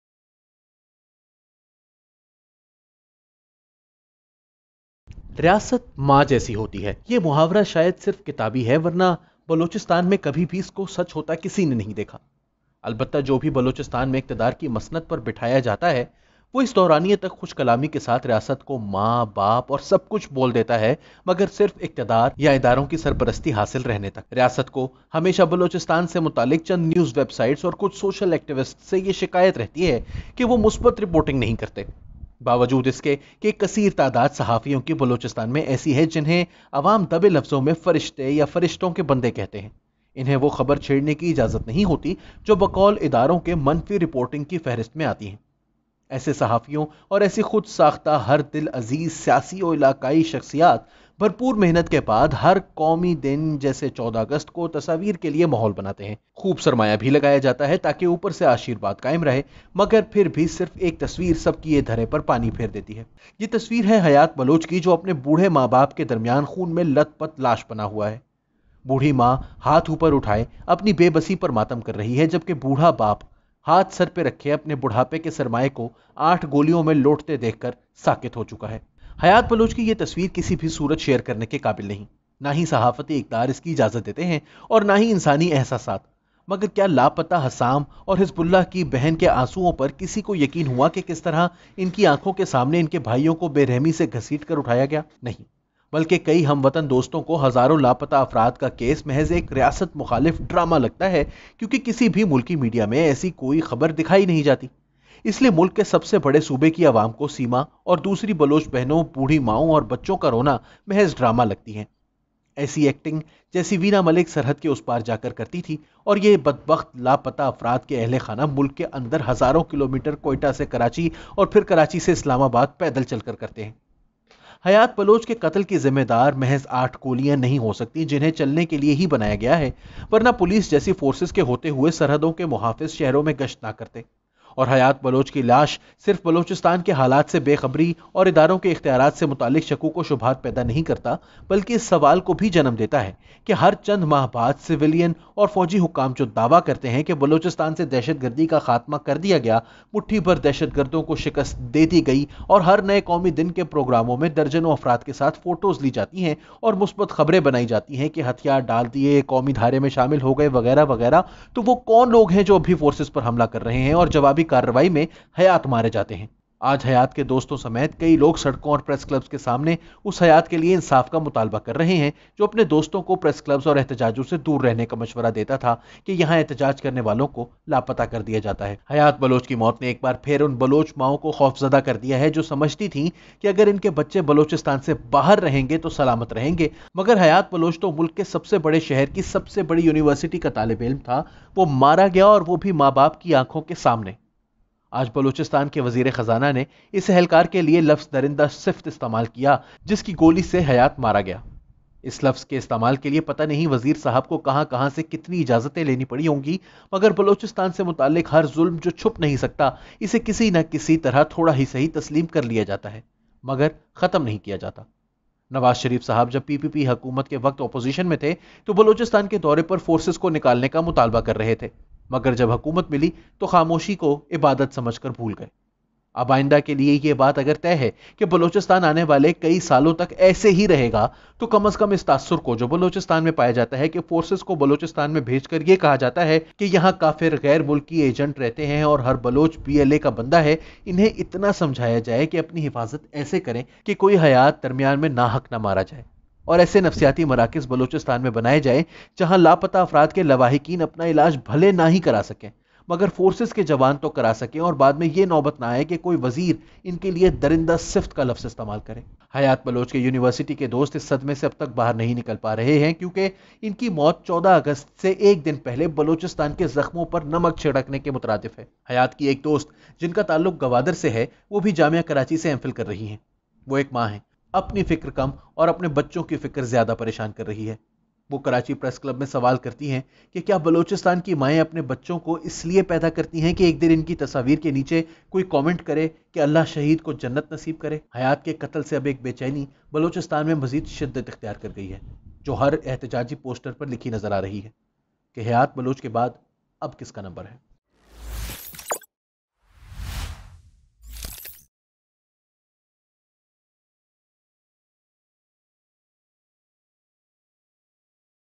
Hayat Baloch, And The Culture Of Impunity
Hayat Baloch incident has left the country in shock. Although the FC official responsible for the act has confessed, the institutional problem is very much there and it'll only be solved once the personnel stop treating every Baloch citizen like a suspect.